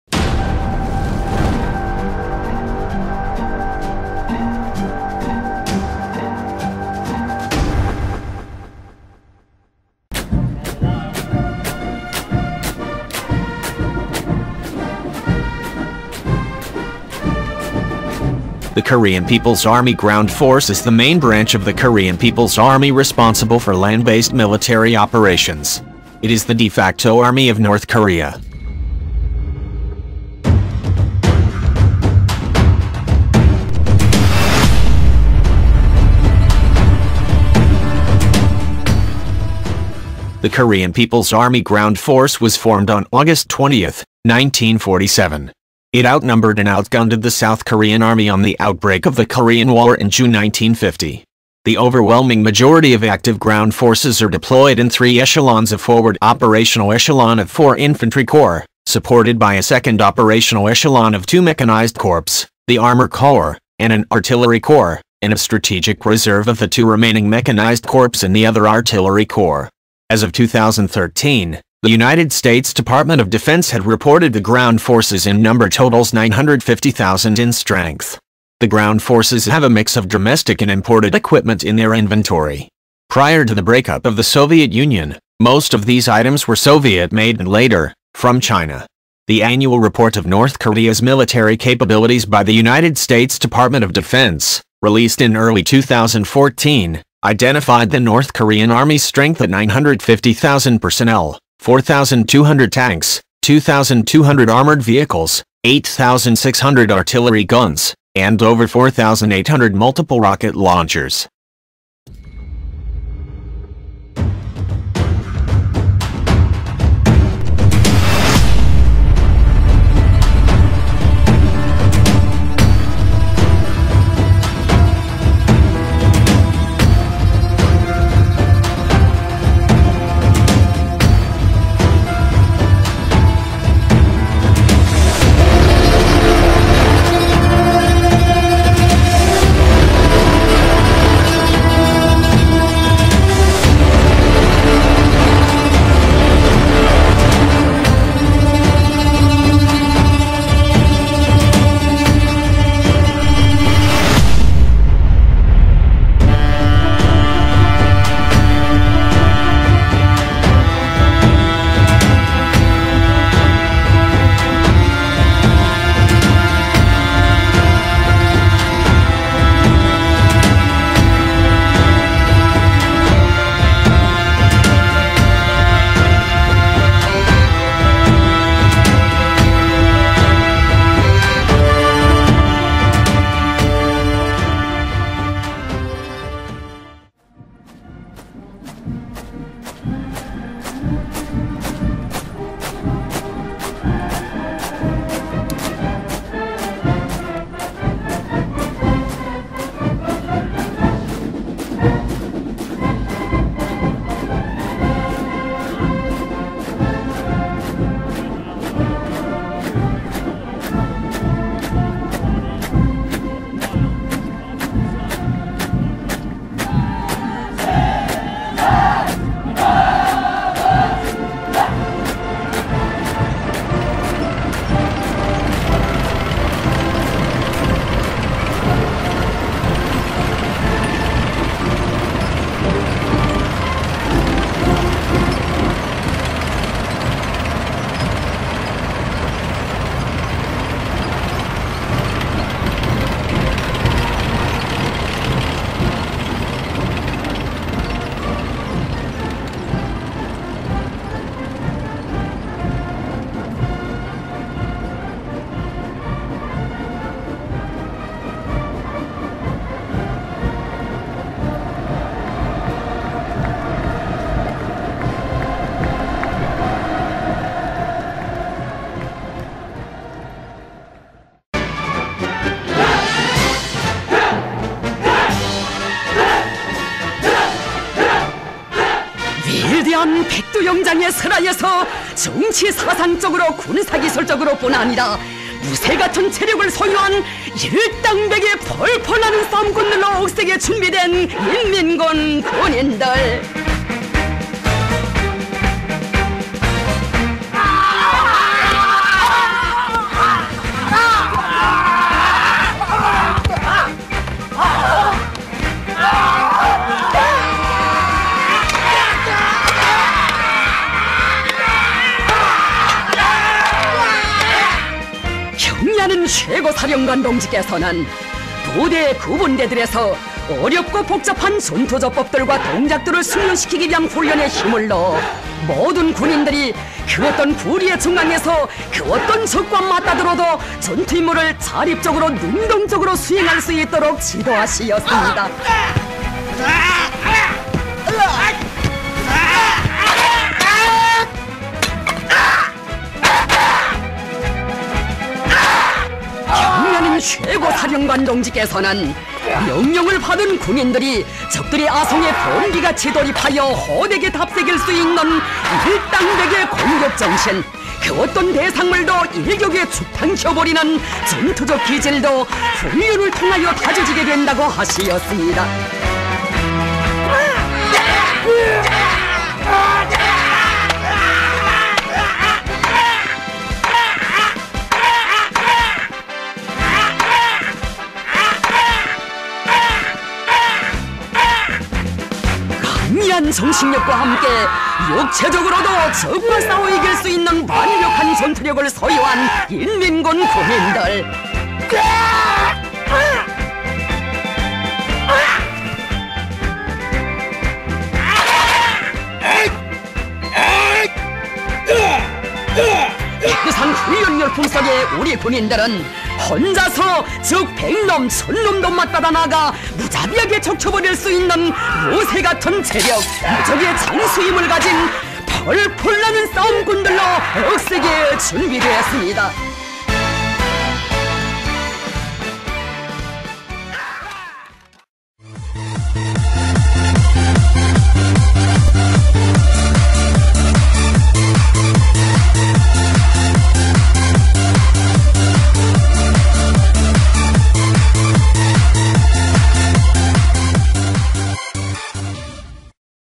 Just How Powerful is NORTH KOREAN ARMY
The overwhelming majority of active ground forces are deployed in three echelons — a forward operational echelon of four infantry corps; supported by a second operational echelon of two mechanized corps, the armor corps, and an artillery corps; and a strategic reserve of the two remaining mechanized corps and the other artillery corps.

These forces include the 806th and 815th Mechanized Corps and the 820th Armored Corps. These forces are garrisoned along major north-south lines of communication that provide rapid, easy access to avenues of approach into South Korea. The KPAGF has positioned massive numbers of artillery pieces, including some fakes,[citation needed] especially its longer-range systems, close to the Demilitarized Zone (DMZ) that separates the two Korea's.